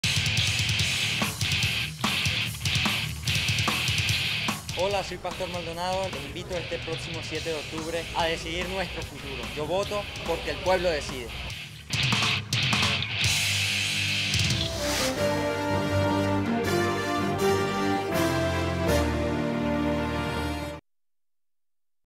Tves- Pastor Maldonado te invita a votar este 07 de octubre.
El piloto de Fórmula 1 Pastor Maldonado,  te invita a decidir nuestro futuro este próximo 07 de octubre  de 2012.